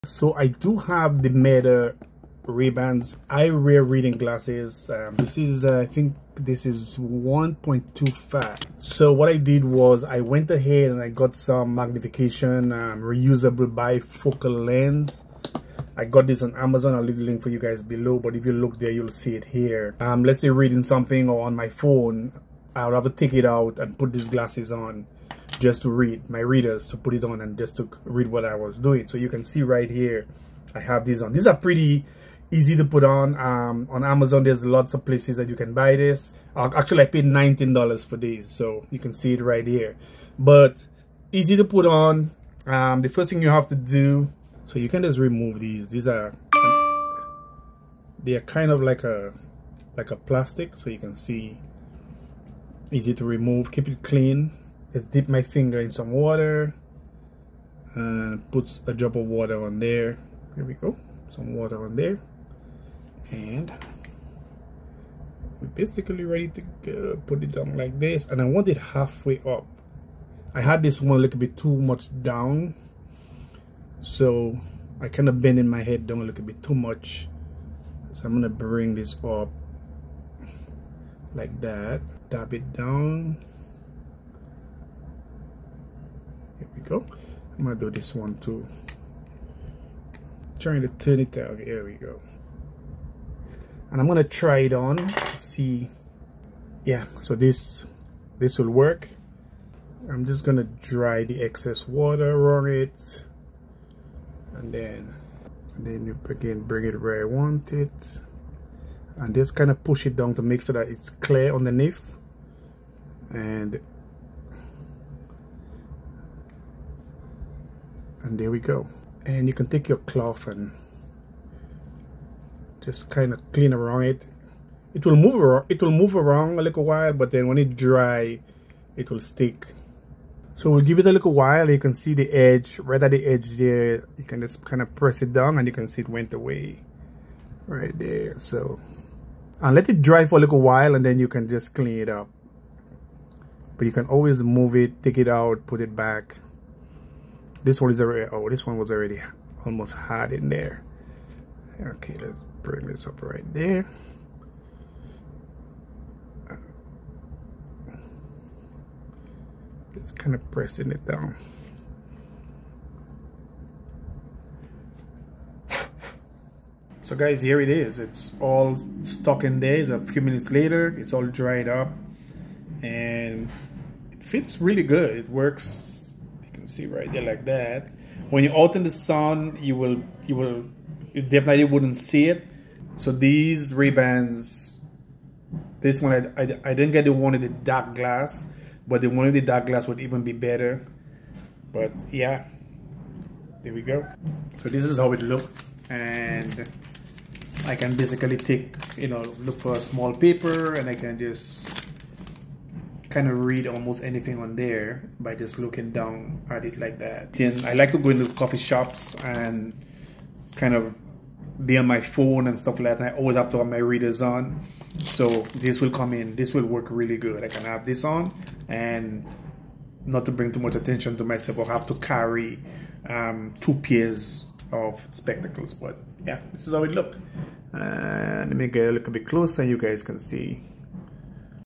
How To Install Stick-On Bifocal Reading Lenses To Smart Shades, Meta Ray Ban
Hydrotac Stick-on Bifocal Reading Glasses

Ray-Ban Meta Wayfarer Smart glasses